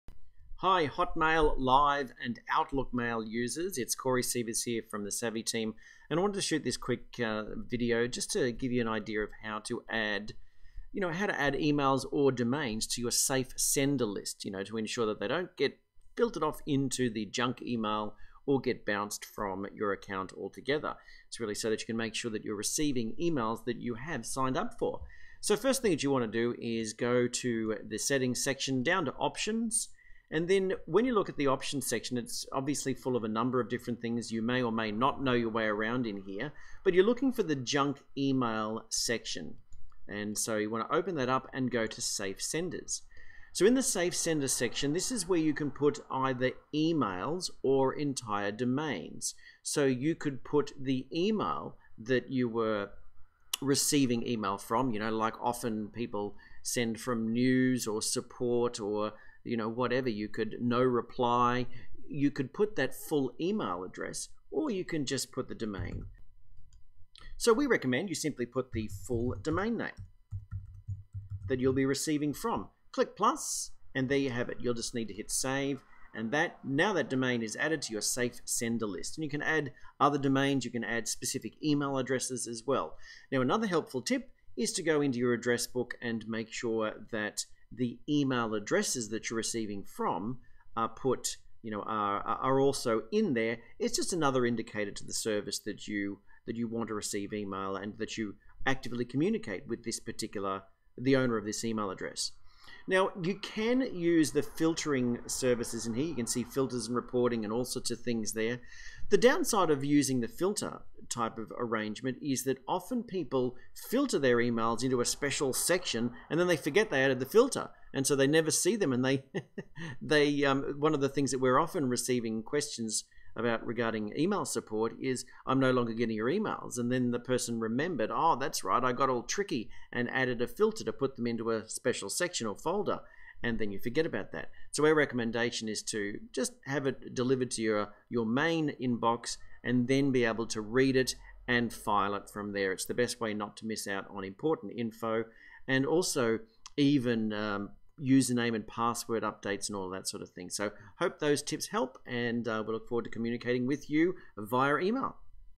How to add to 'Safe Sender' list - Outlook, Homail, Live
In this video you’ll learn how to add a domain or email to the ‘Safe Sender’ list for Outlook / Hotmail / Live users - so the emails you are subscribed to are more likely to reach your inbox!